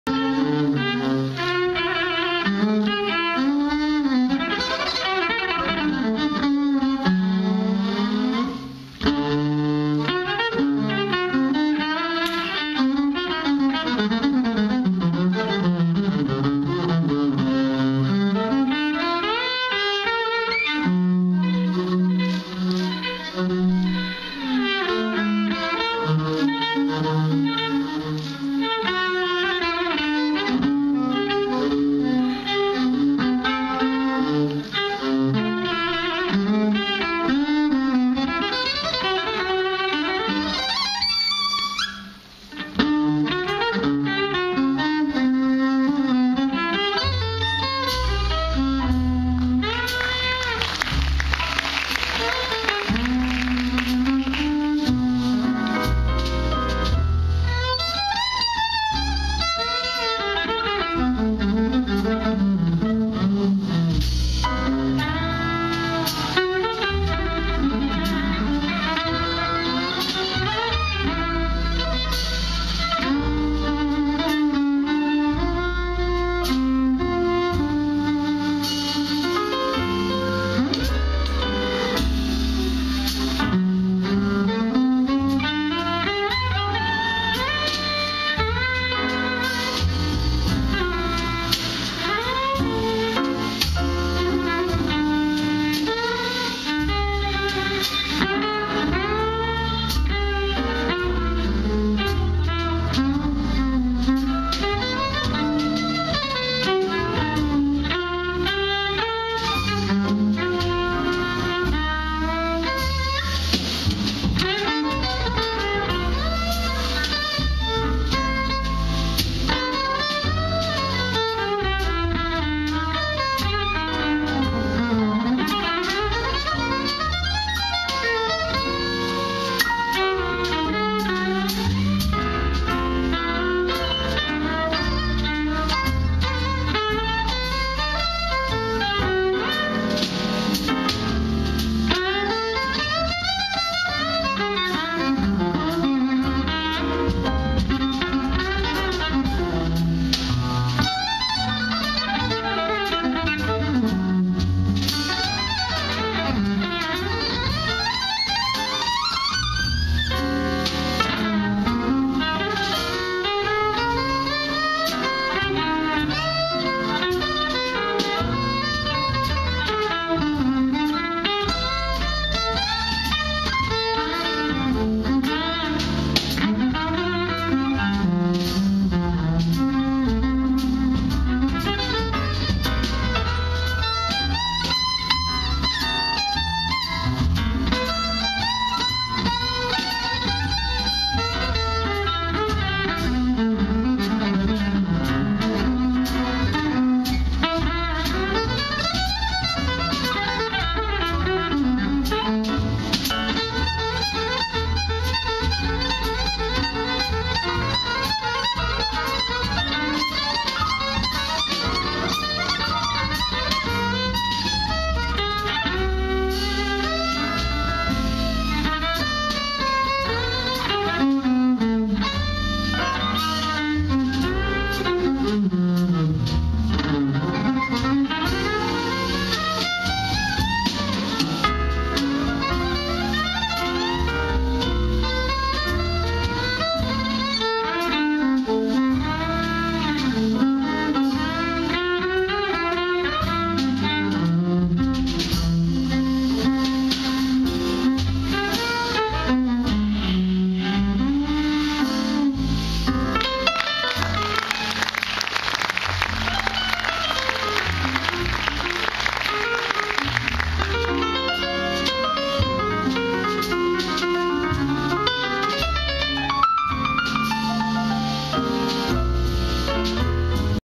Terri Lynne Carrington, Zack Brown, Michal Urbaniak 18.10.2014 Glogów
Razem na Głogowskich Spotkaniach Jazzowych